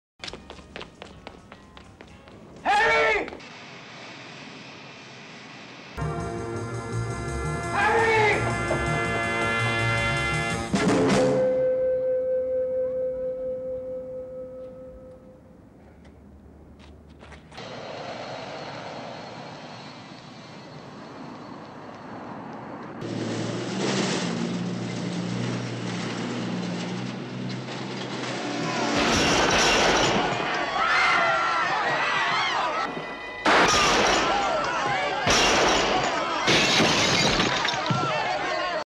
McGill vs. Ross
Mac catches up with the Chicago gangster, chasing him and then killing him in self-defence.